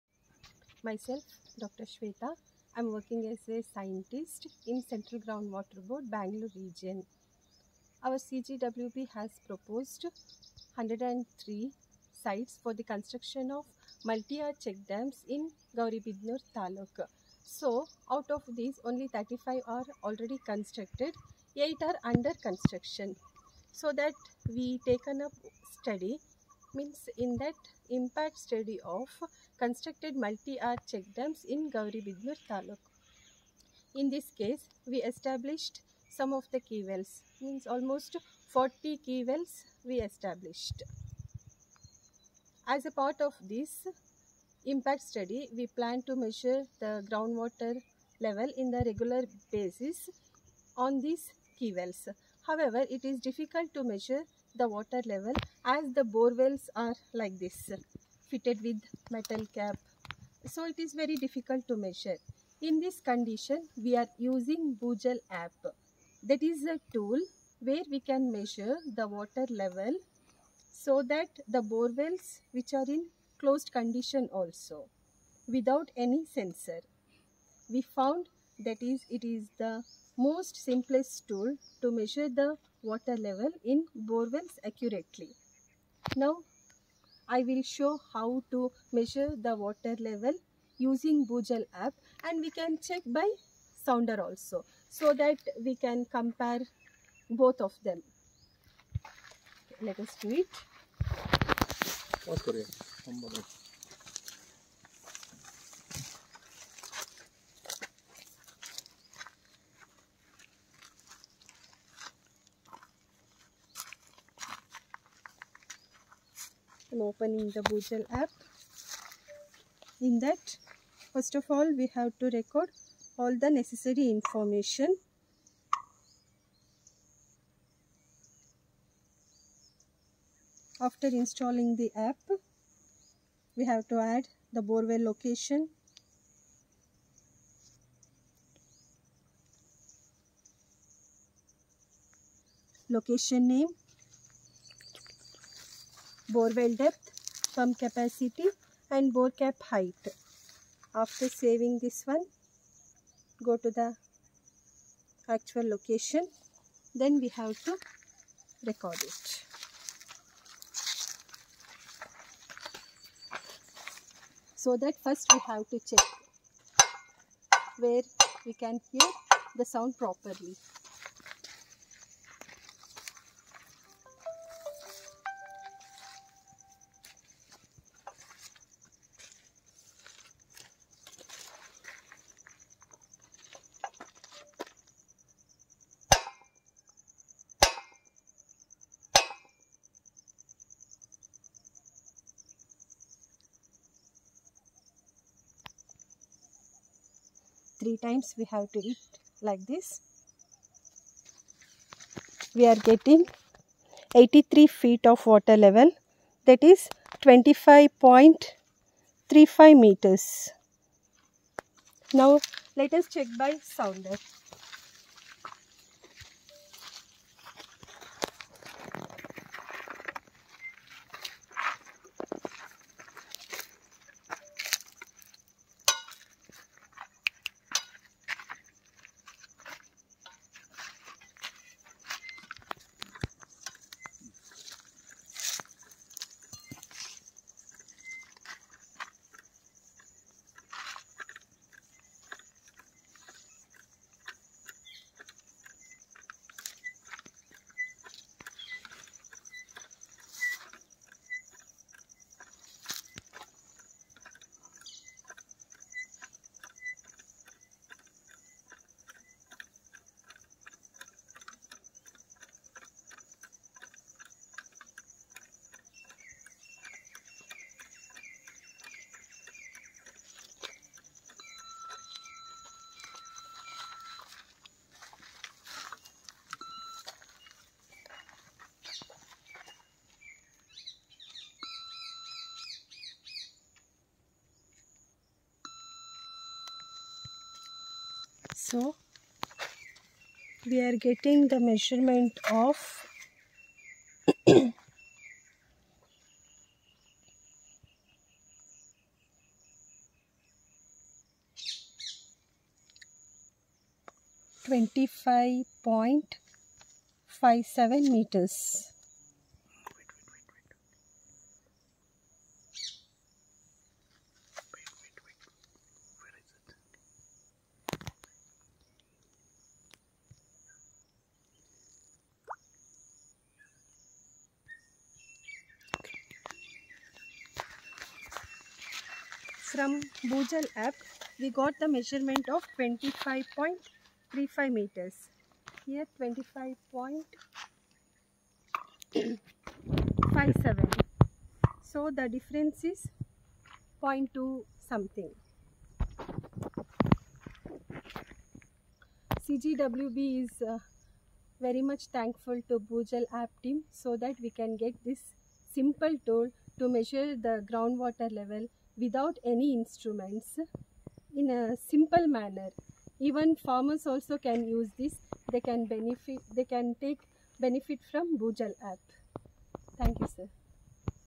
Monitoring of Borewells with CGWB - Gauribidanur Taluk - Well 2
The ground water level in the well has been measured with Bhujal App and with Sounding tape by the CGWB Bengaluru team in the month of December 2023.